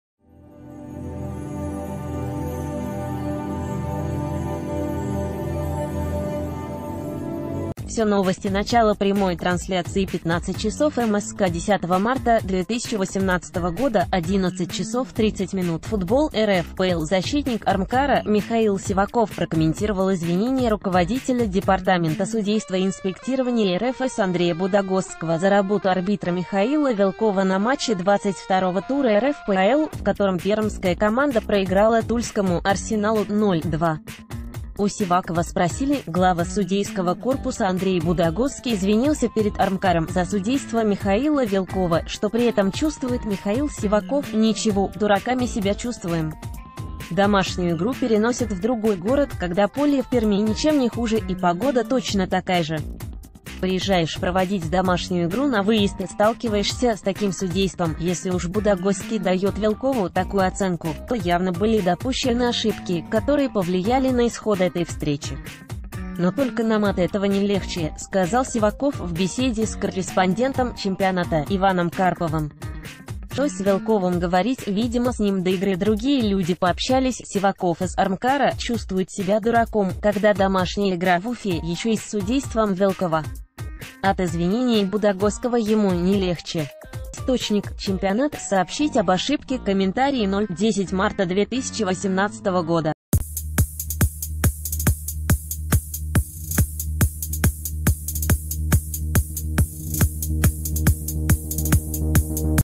Последние новости | Сиваков: после матча «Амкара» с тульским «Арсеналом» чувствуем себя дураками
Все новости Начало прямой трансляции 15:00 Мск 10 марта 2018, 11:30 Футбол / РФПЛ Защитник «Амкара» Михаил Сиваков прокомментировал извинения руководителя департамента судейства и инспектирования РФС Андрея Будогосского за работу арбитра Михаила Вилкова на матче 22-го тура РФПЛ, в котором пермская команда проиграла тульскому «Арсеналу» (0:2). У Сивакова спросили: «Глава судейского корпуса Андрей Будогосский извинился перед «Амкаром» за судейство Михаила Вилкова. Что при этом чувствует Михаил Сив...